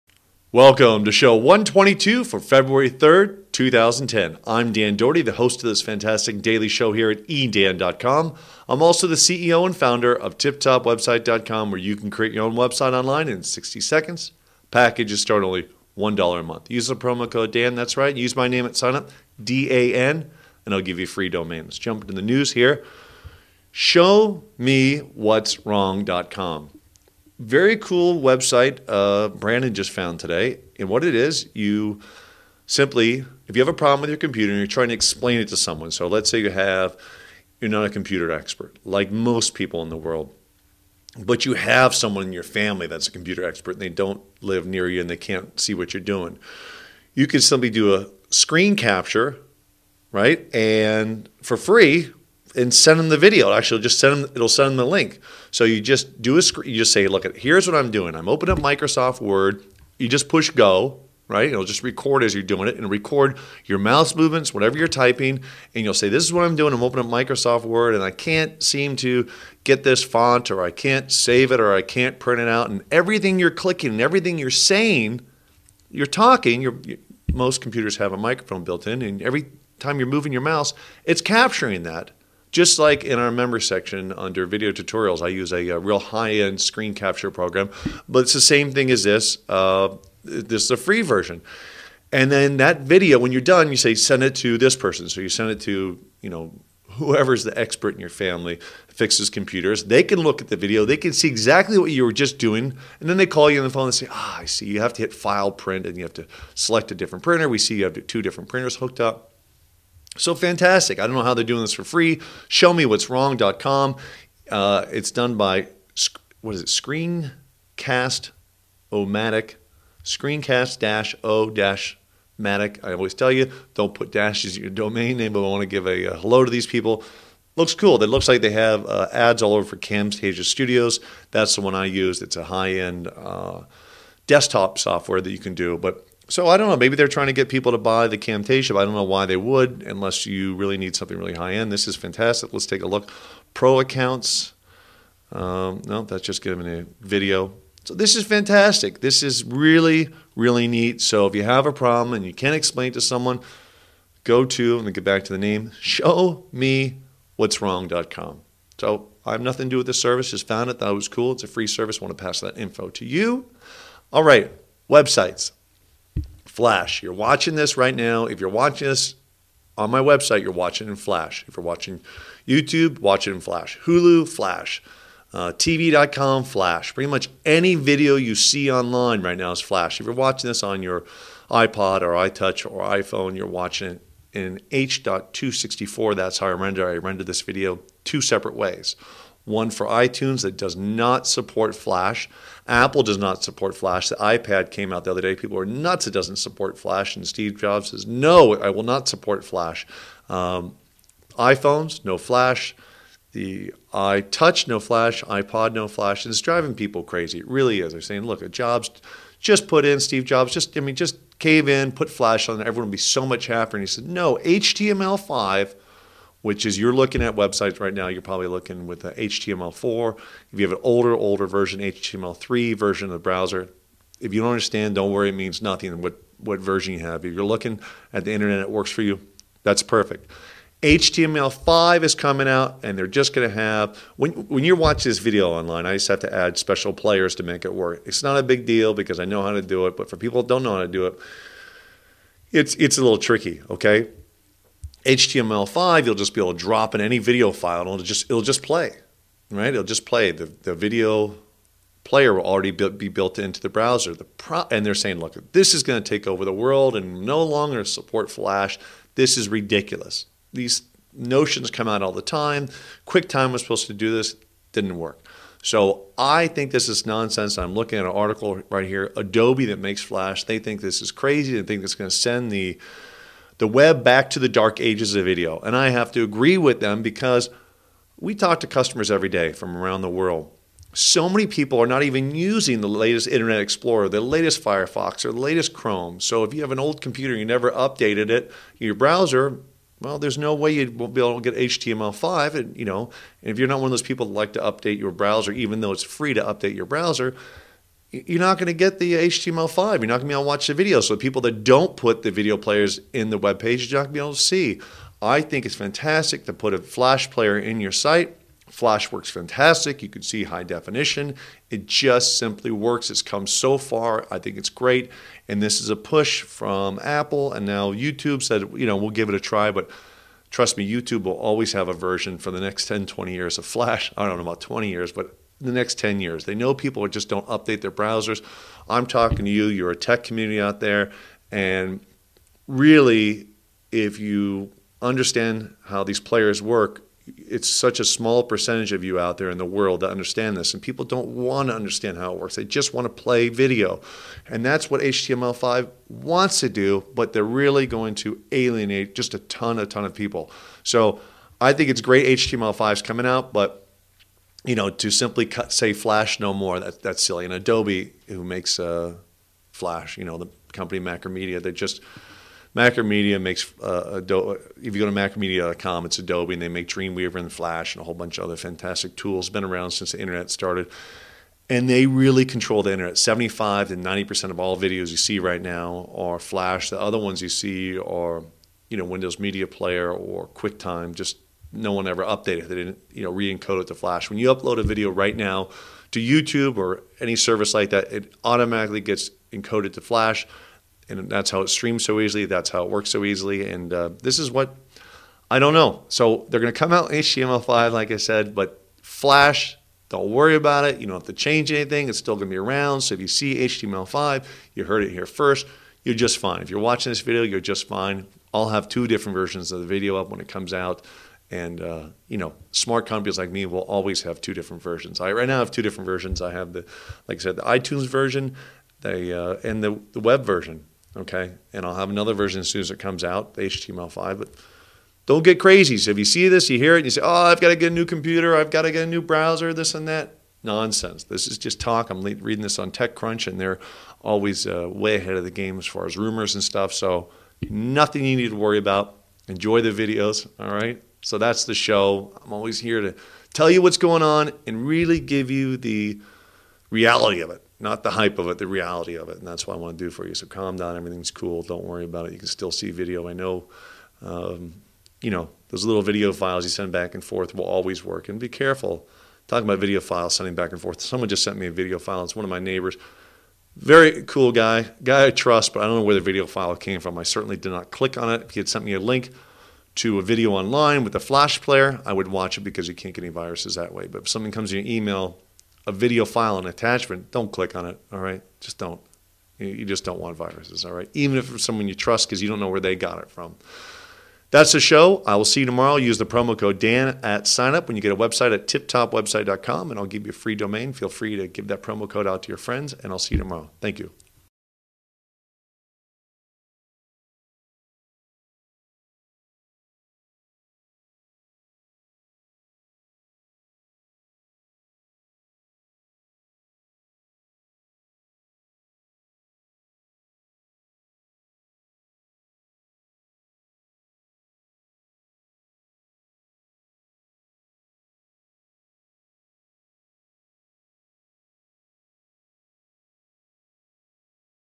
eDan.com Video Show 122
Free Online Screen Video Capture, HTML 5 And Flash - Who Will Come Out On Top?, Free Domain. Hosted by the CEO of TipTopWebsite.com Dan Doherty